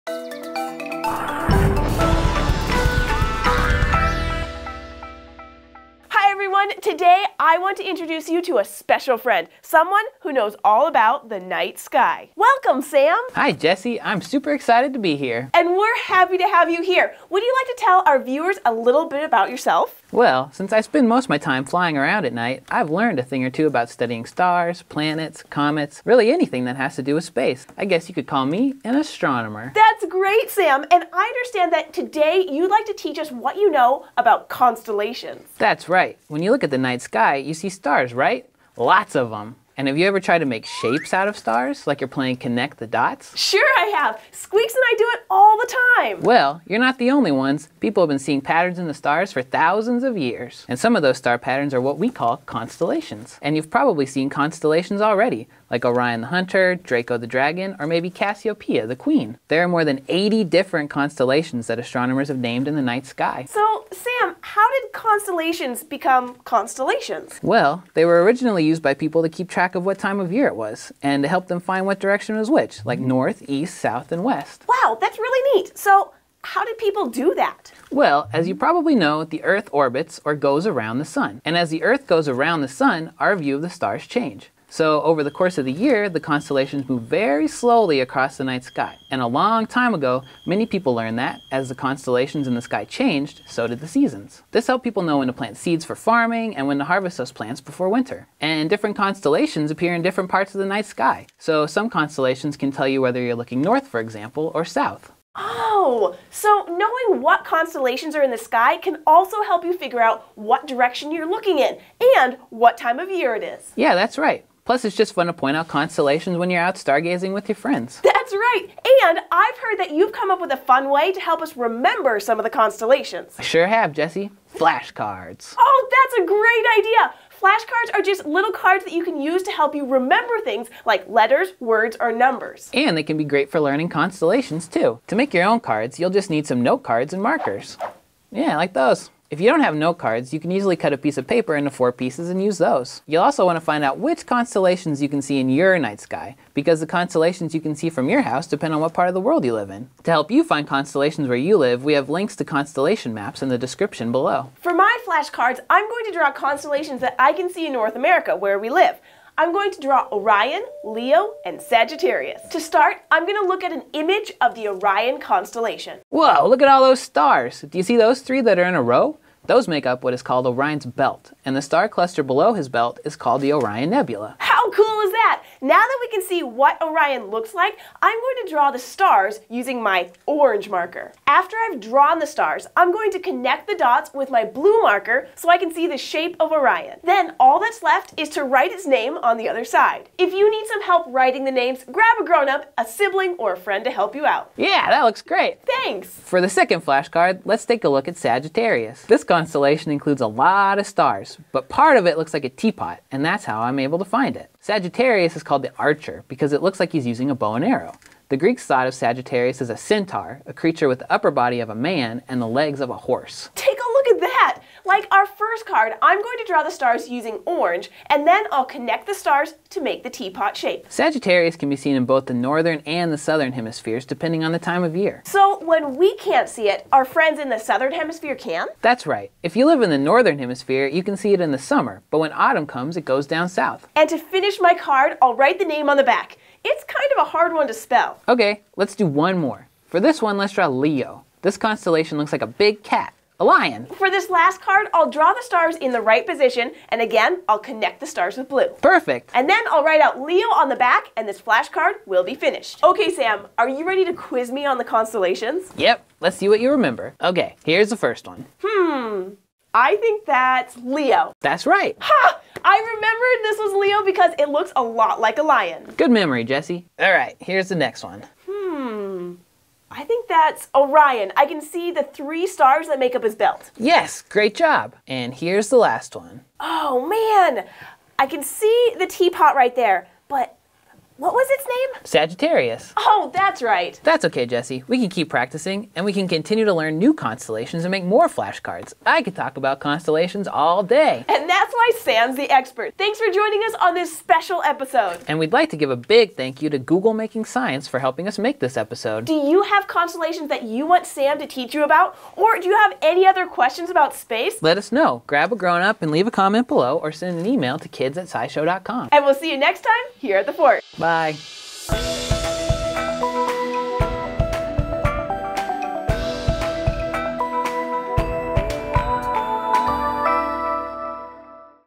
Create Constellation Flash Cards
Jessi has a special guest today who is an expert on the night sky! Join Jessi and Sam the Bat to learn all about constellations, and to test your star knowledge by making flash cards!